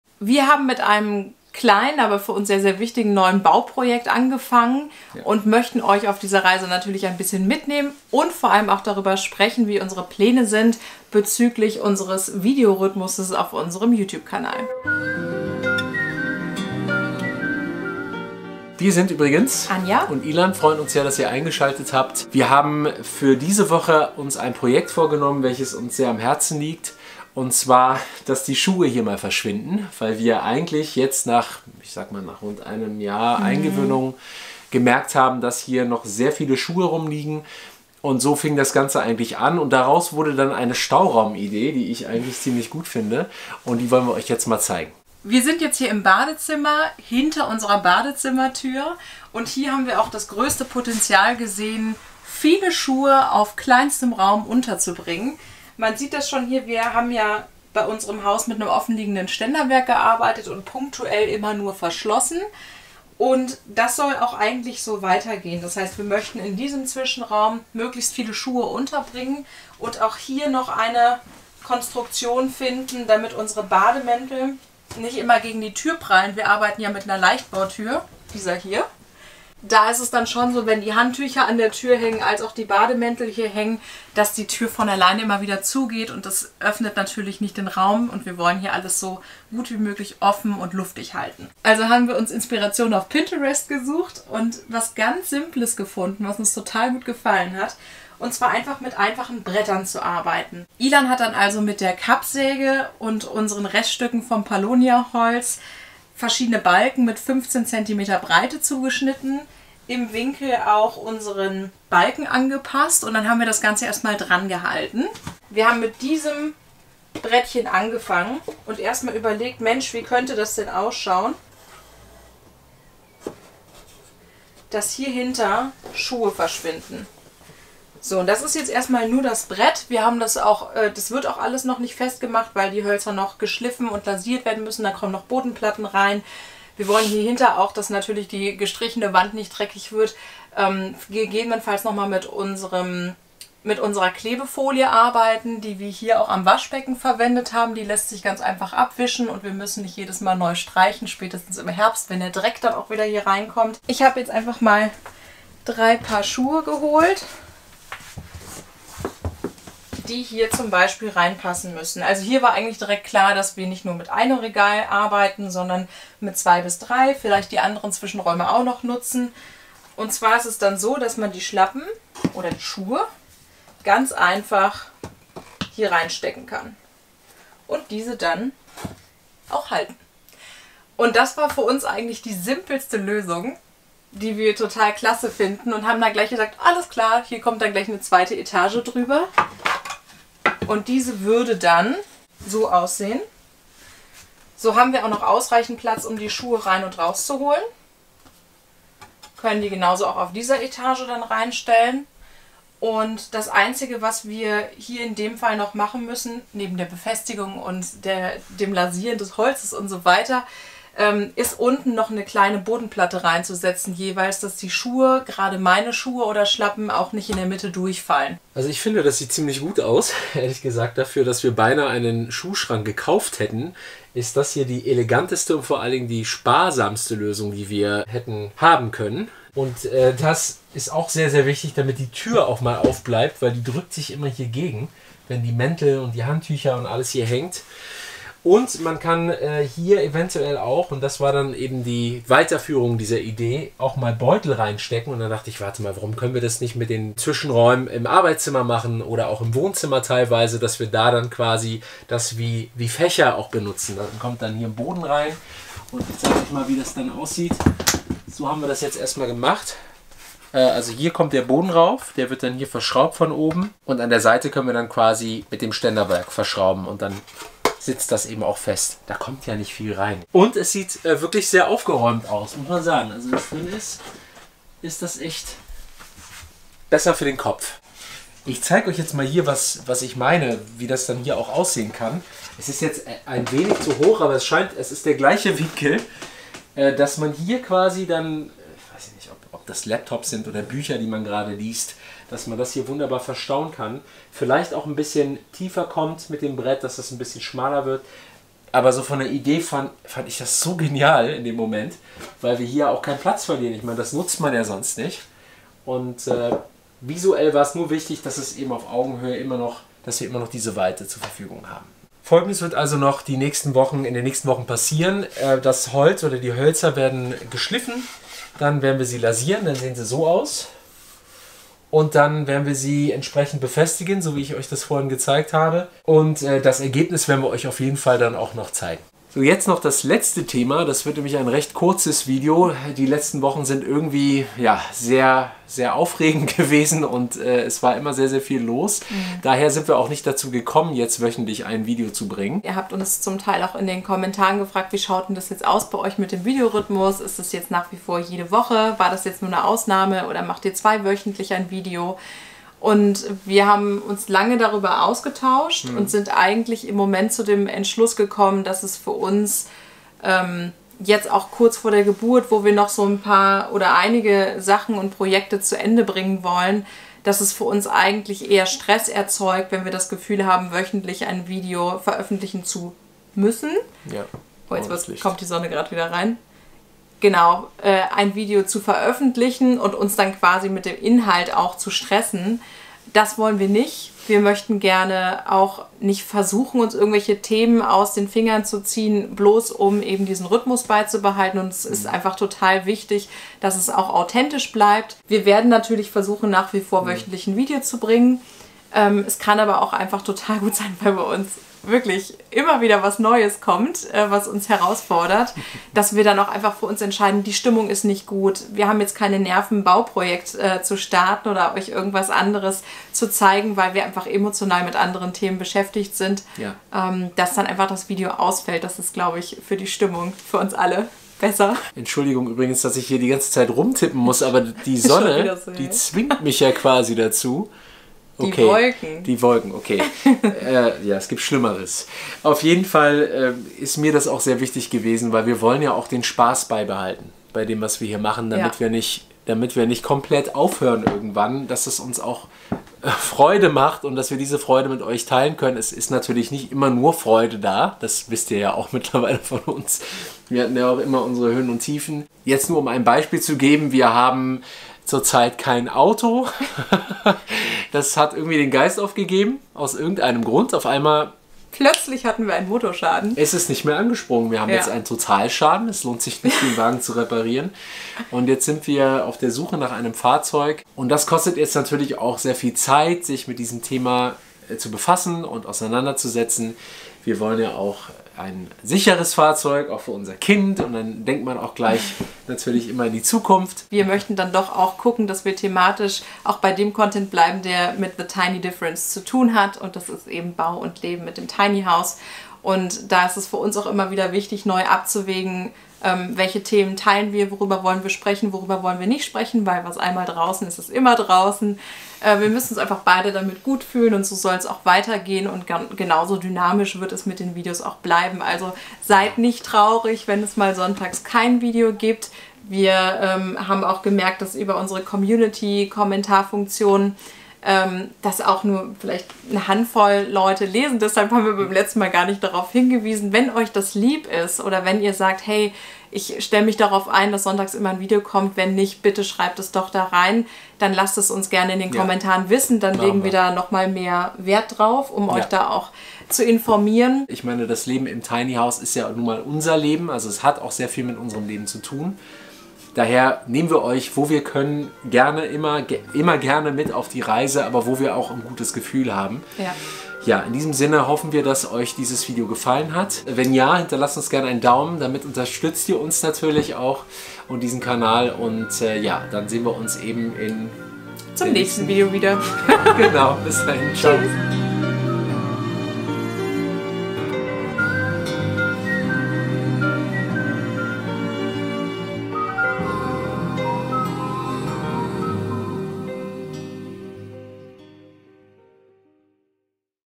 Inspiration für platzsparendes Schuhregal - Fachwerk als Stauraum + künftige Videos
Unsere Schuhe stehen immer im Weg, weshalb ein platzsparendes Schuhregal entstehen soll. Wir zeigen unsere ersten Ideen und sprechen über die Taktung unserer künftigen Videos.